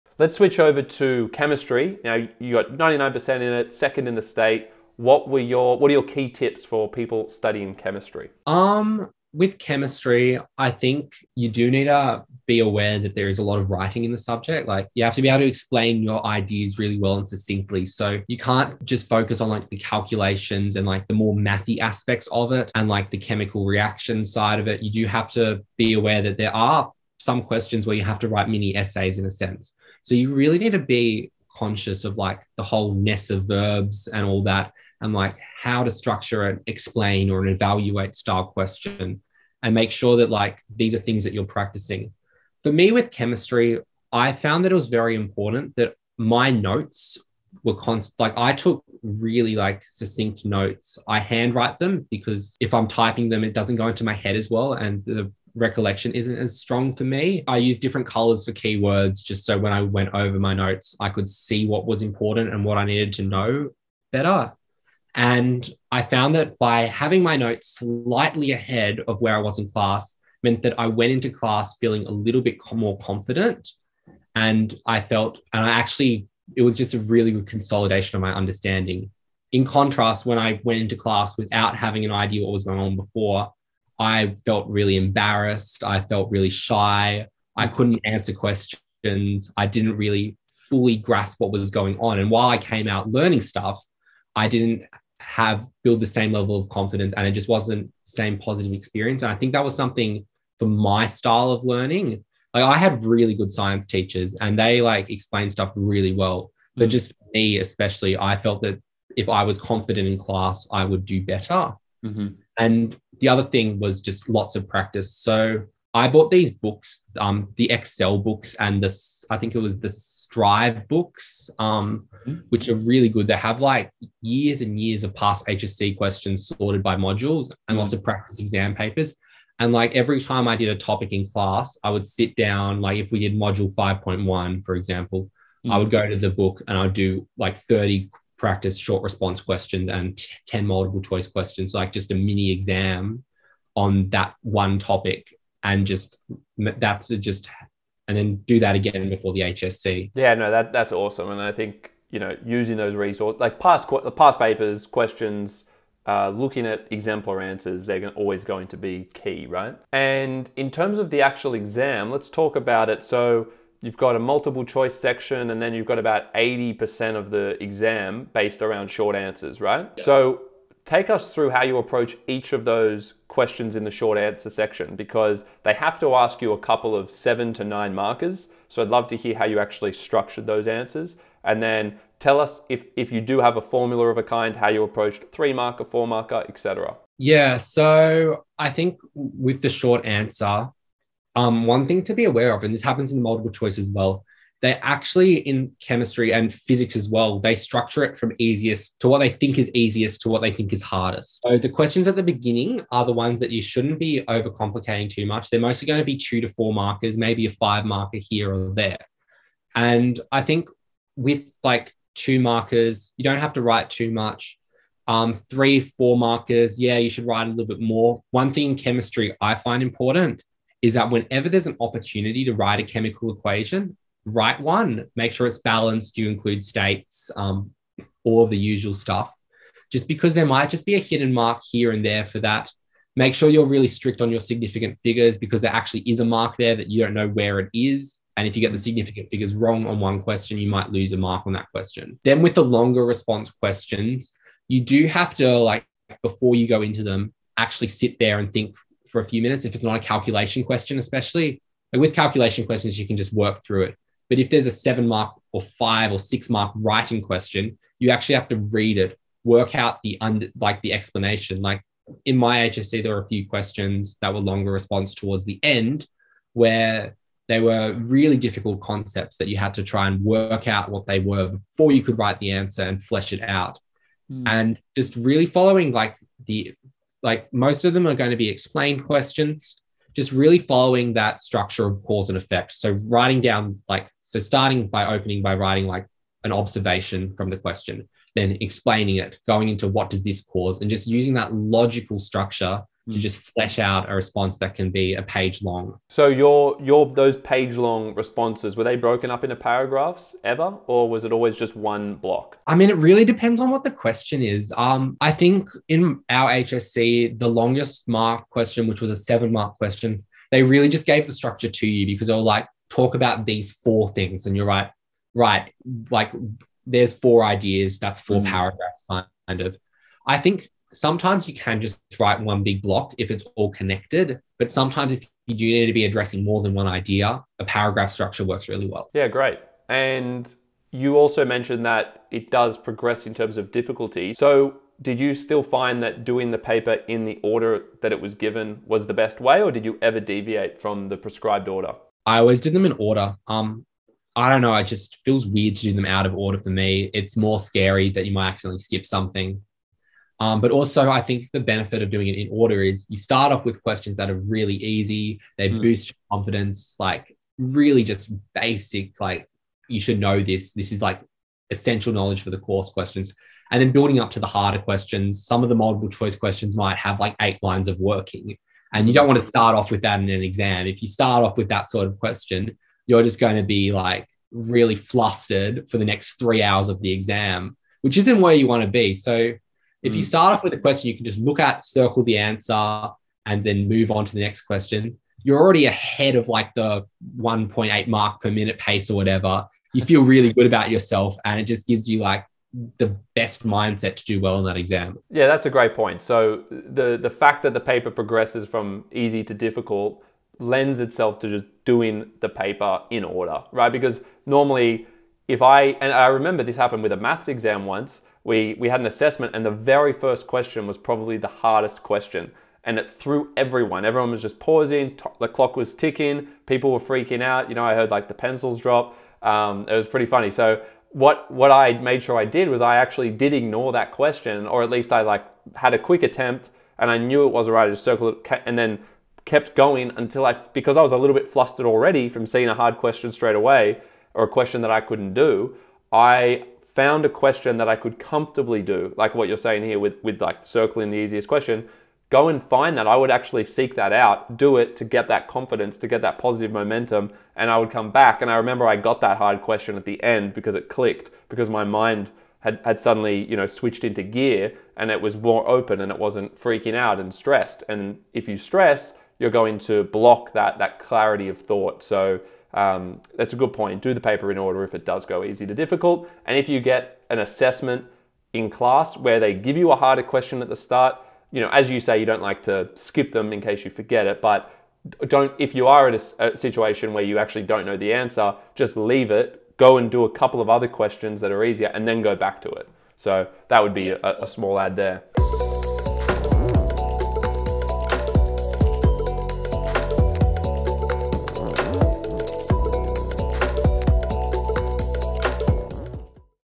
Chemistry Tips from a HSC State Ranker
Jack completed Year 12 in 2021 with a perfect ATAR of 99.95 and placed 2nd in NSW for chemistry with a final mark of 99%. Find out his top tips for chemistry in this video.